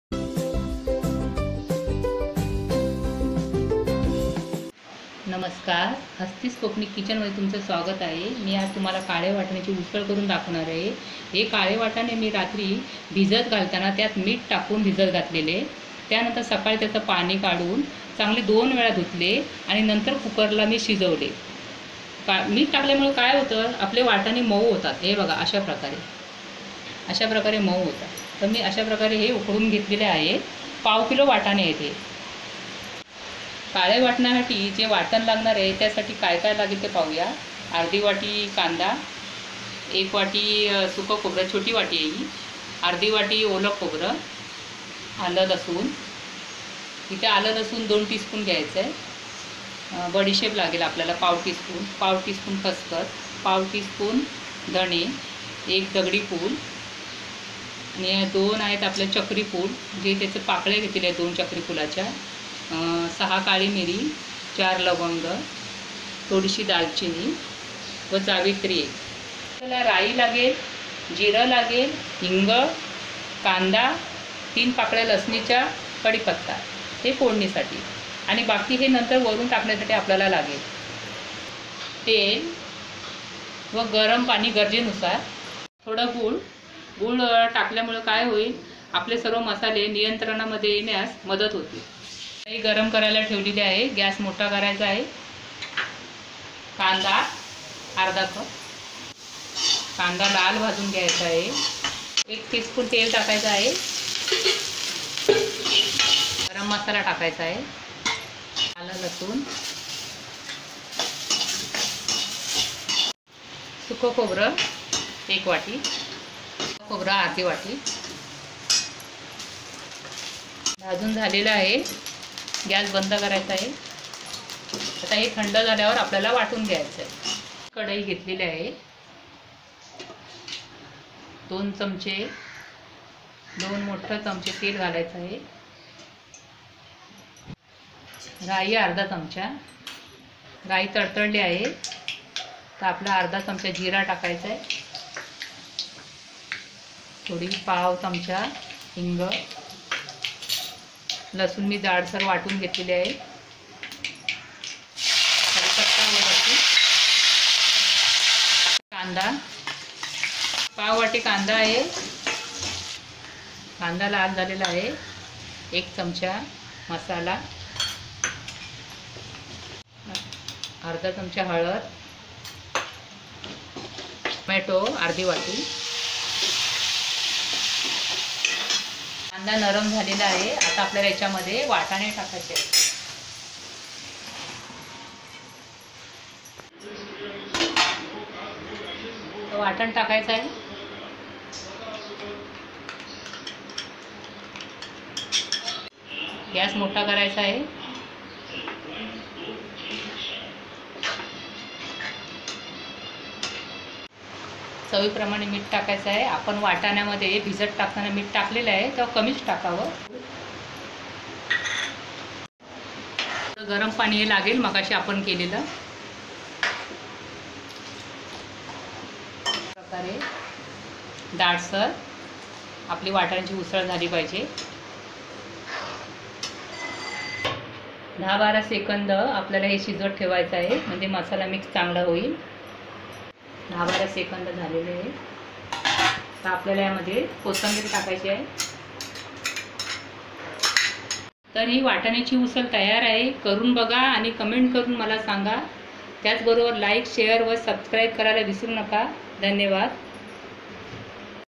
झणझणीत काळ्या वाटाण्याची उसळ रेसीपी | Hasti'S Kokani Kitchen | Recipe in Marathi | Kokani Tadka
Hasti'S Kokani Kitchen तुमच्यासाठी घेऊन आले आहे काळ्या वाटण्याची झणझणीत भाजी / उसळ रेसीपी..
तुम्ही ही रेसीपी घरी नक्की करून पहा. आणि तुमच्या प्रतिक्रिया आम्हाला जरूर कळवा.
If U like our video then please do ,
Stay Healthy .